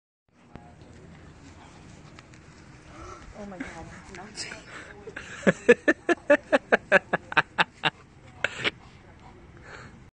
IS THIS LEGAL to deer hunt like this with a dog?
A Bull Mastiff wanted some deer meat